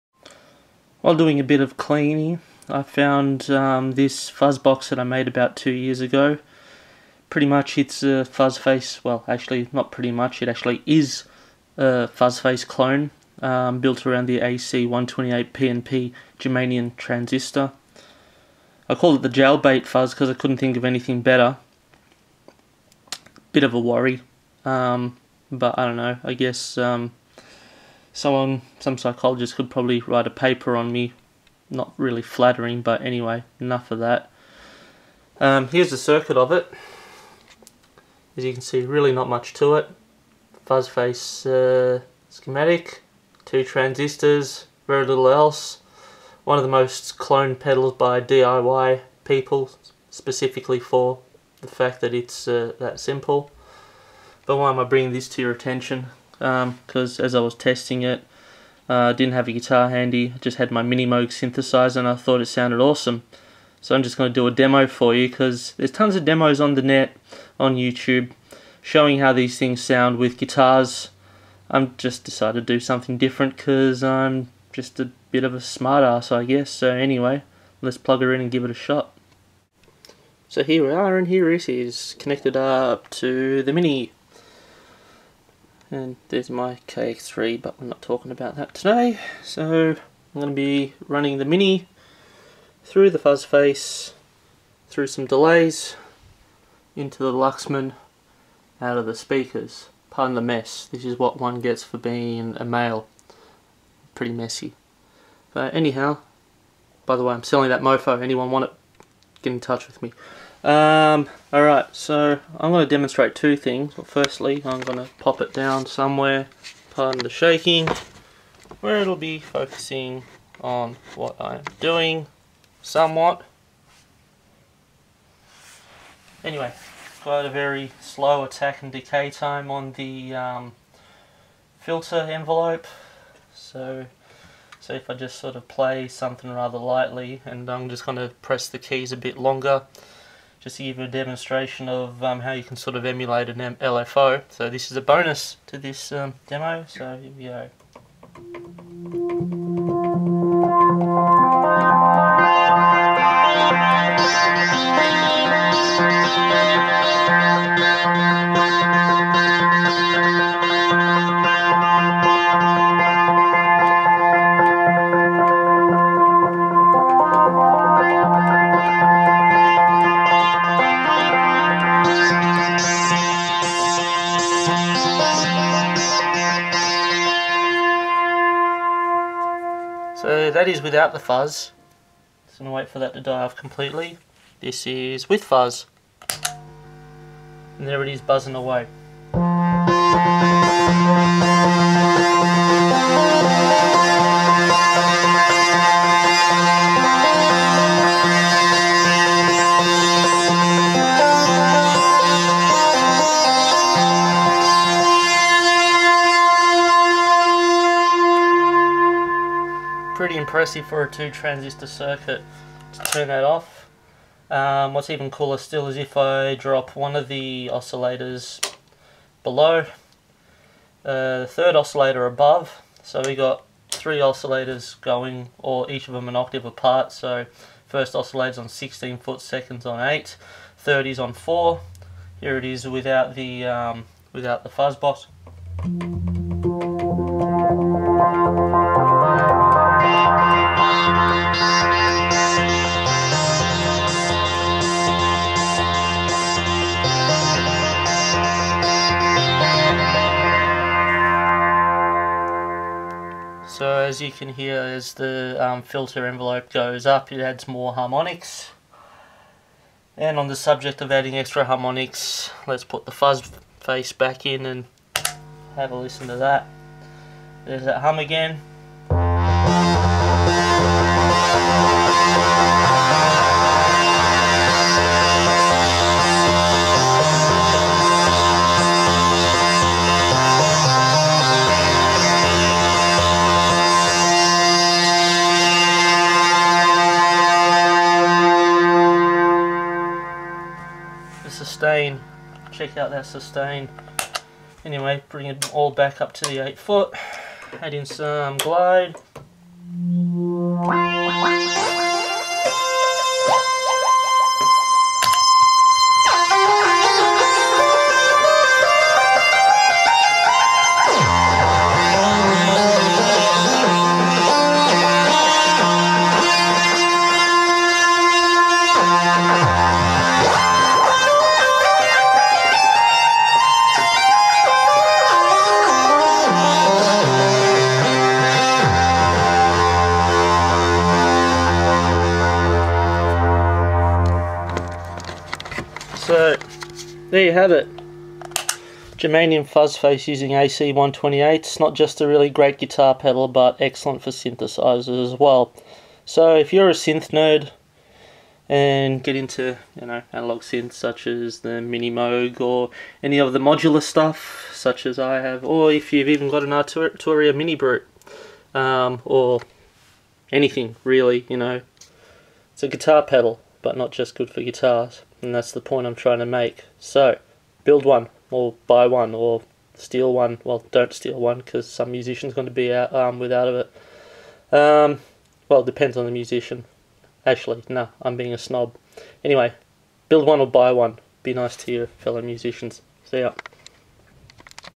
Germanium Fuzz Face With A Minimoog
A demonstration of a homebuilt Fuzz Face, a guitar fuzz pedal, with a 1973 Minimoog synth. The pedal utilises a pair of AC128 germanium PNP transistors.

This video came about after finding the pedal in my room and trying it out on the synth. The combination sounded great so I decided to make a video to show it being used on something other than a guitar.

The Fuzz Face was made famous by such guitarists as Jimi Hendrix and David Gilmour. Although the one demonstrated uses germanium transistors, later versisons used silicon transistors such as the BC108 which had more gain and a buzzier sound. Yes, I made one of those too.